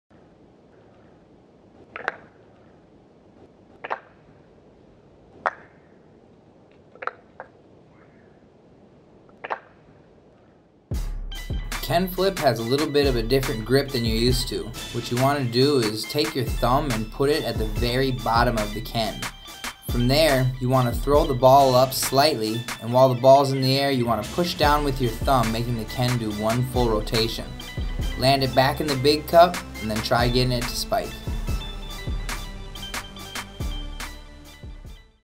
Kenflip | Sweets Kendamas | Tips & Tricks
Tips & Tricks is a video tutorial series designed to progress any player's technique, whether they a beginner or a pro!

Kenflip:

Kenflip has a little different grip that you're used to.

What you want to do is take your thumb and put it at the very bottom of the ken, from there you want to throw the ball up slightly, and while the ball is in the air you push down on the bottom of the ken making it do a full flip.

Land it back in the big cup and then proceed to spike.